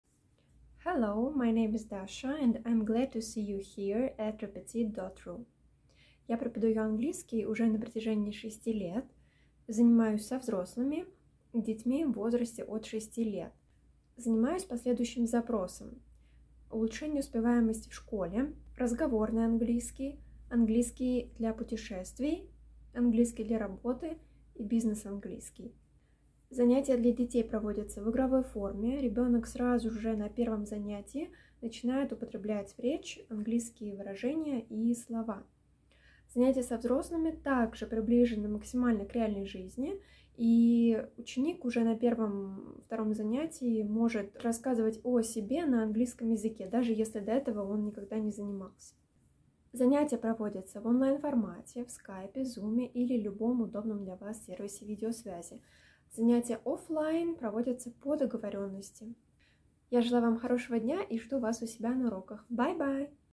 Мешкова Дарья Германовна - репетитор по английскому языку - видеопрезентация для Repetit.ru
Видеопрезентация репетитора по английскому языку Мешковой Дарьи Германовны для REPETIT.RU.

Мешкова Дарья Германовна окончила Общество с ограниченной ответственностью "Наука" (Москва).